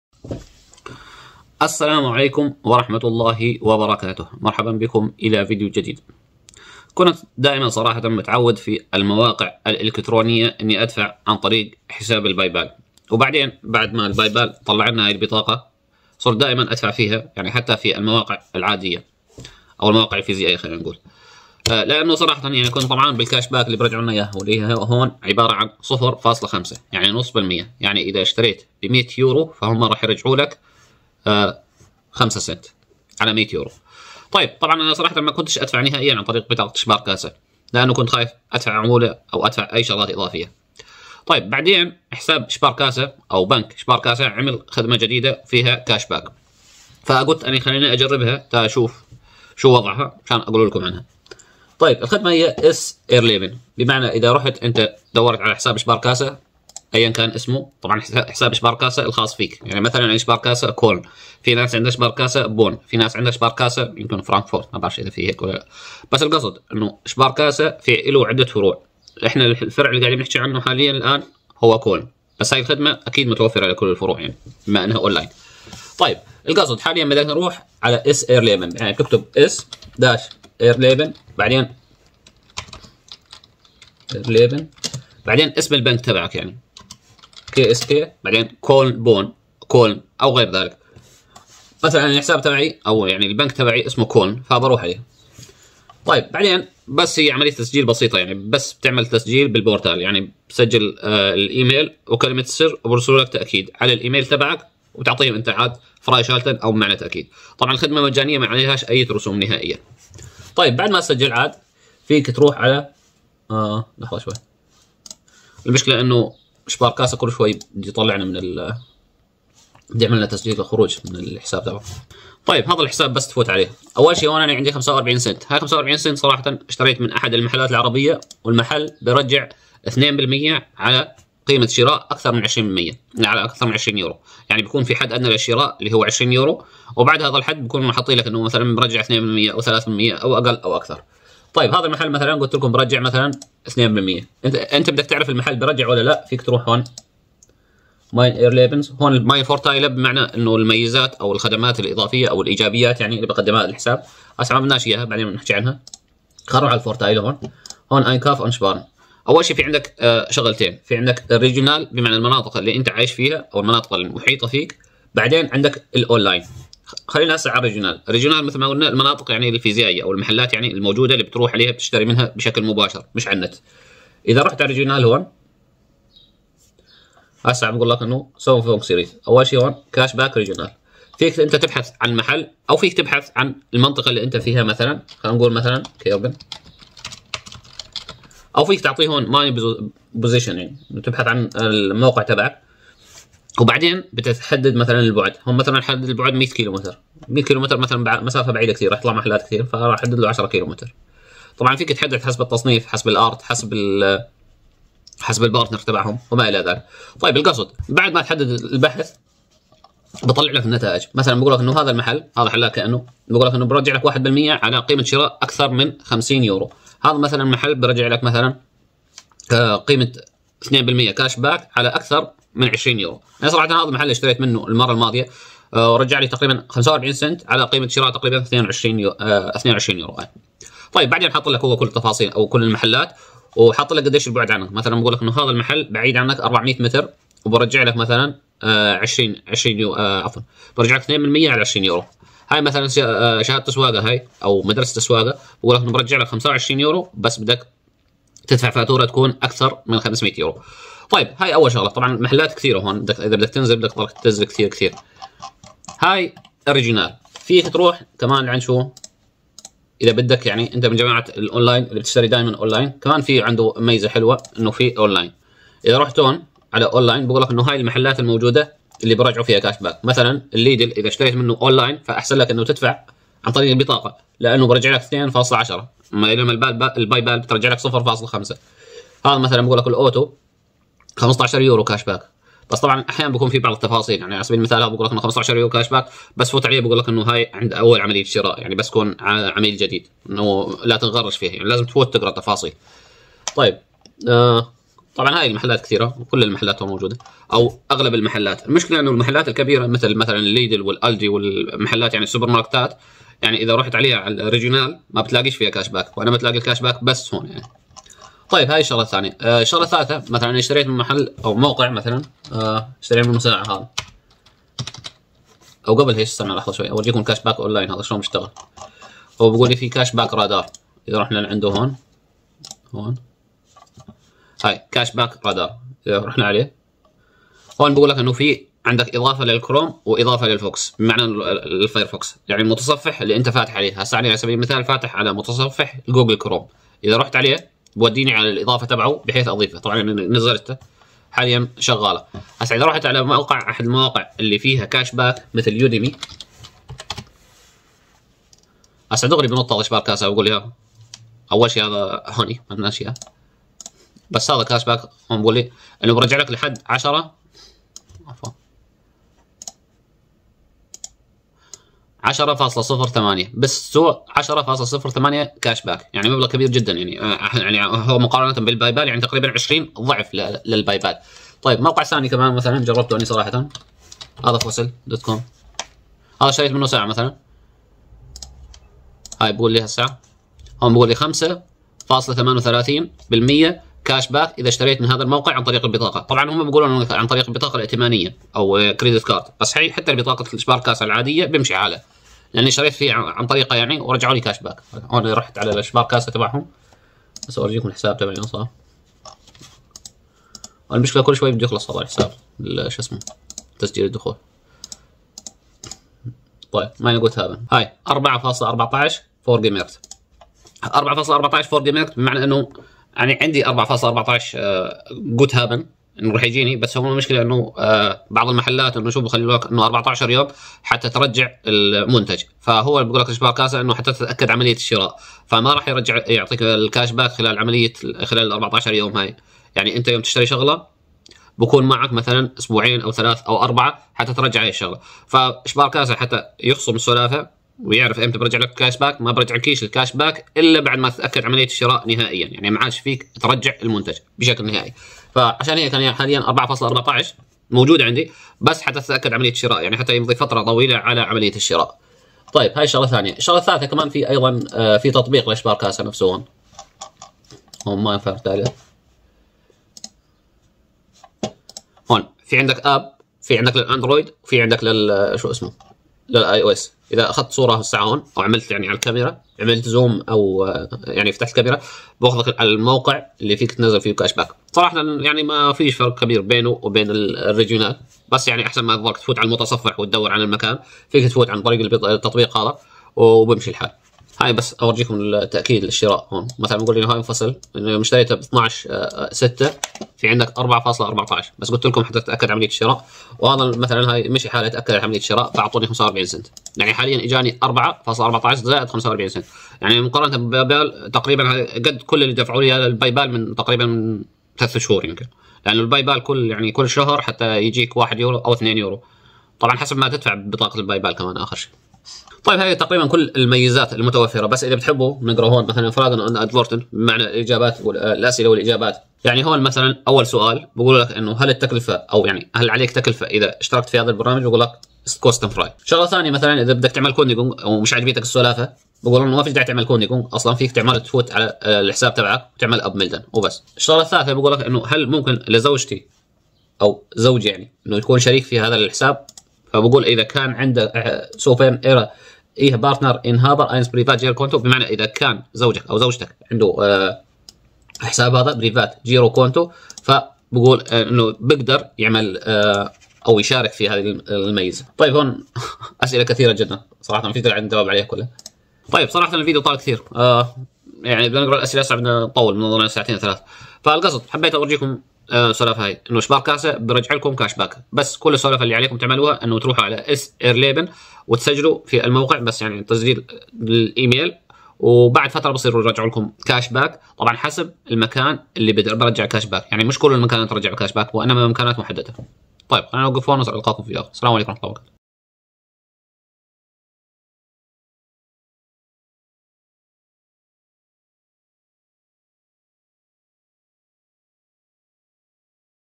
شباركاسة يفعل خدمة كاش باك لغاية 10 بالمئة sparkasse s-erleben
السلام عليكم
قام شباركاسة اخيراً بتفعيل ميزة الكاش باك حيث سيمكنك أخيراً من الحصول على استرجاع نقدي عند دفع حد أدنى من النقود لكن هذه الخدمة ليست مفعلة في كل المناطق وانما في بعض المناطق المعينة وفي بعض الصفحات المعينة
يمكنك معرفة ذلك بعد أن تقوم بالتسجيل بالموقع